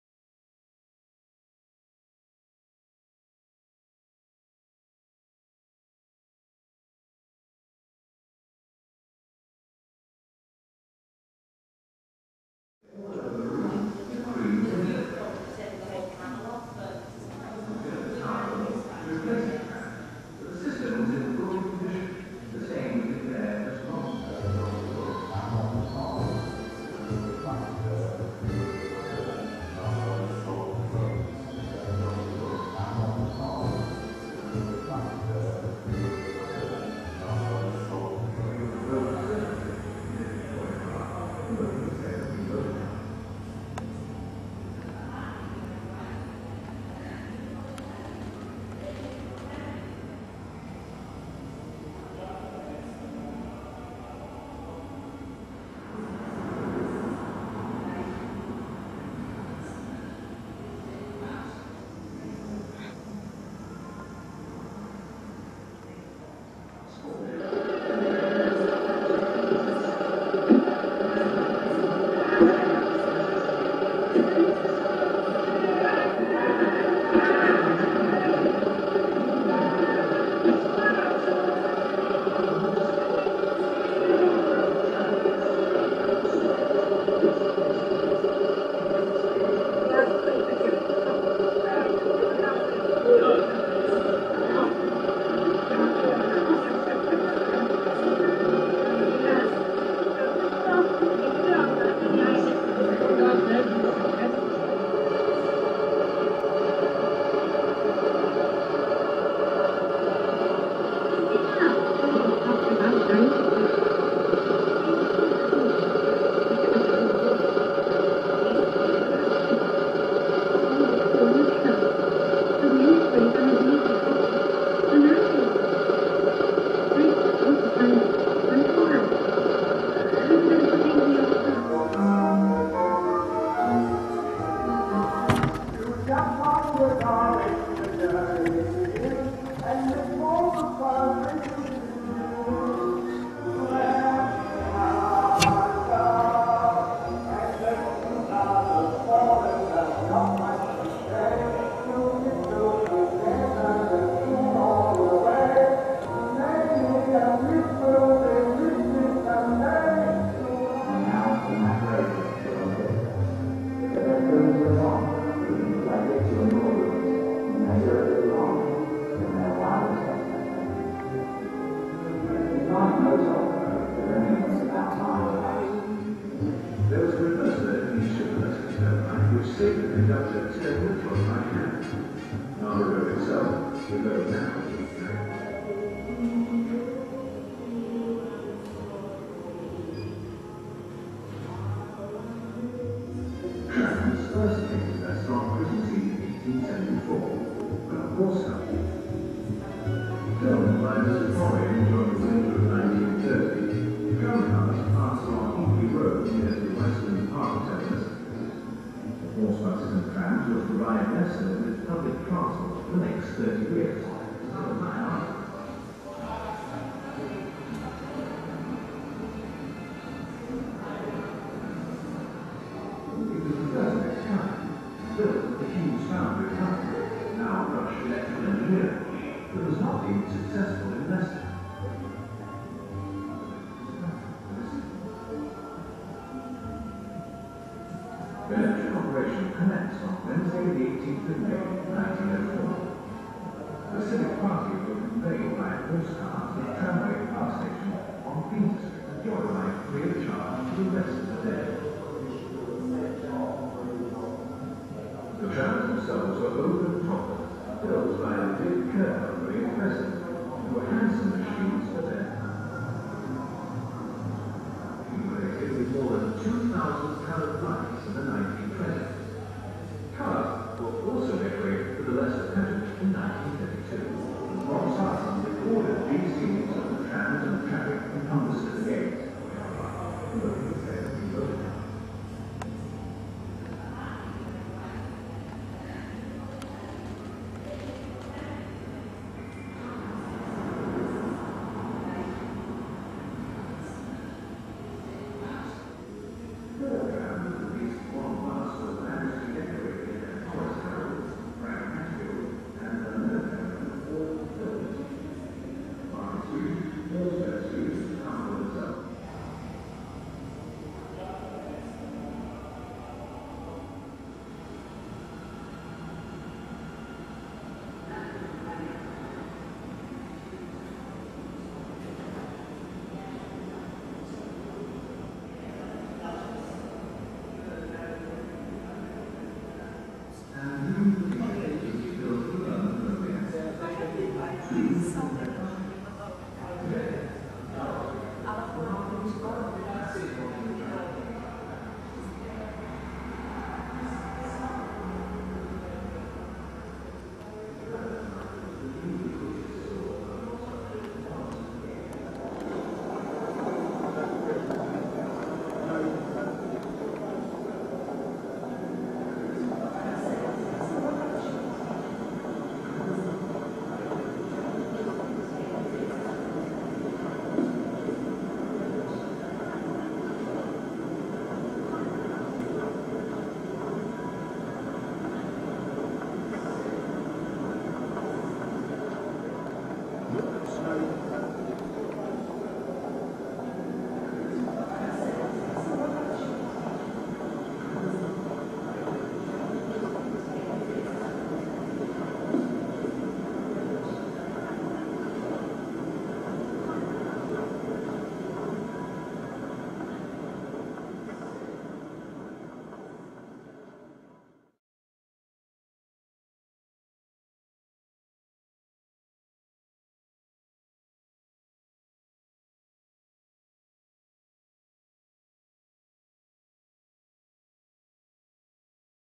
Abbey Pumping Station Museum - Inside.
Here we take a look at the inside of the museum. This area shows some of the relics from the past. The pump house itself is on a separate video.
Filmed by James Hutton. April 28/29th 2010.
MOVING WITH THE PAST films.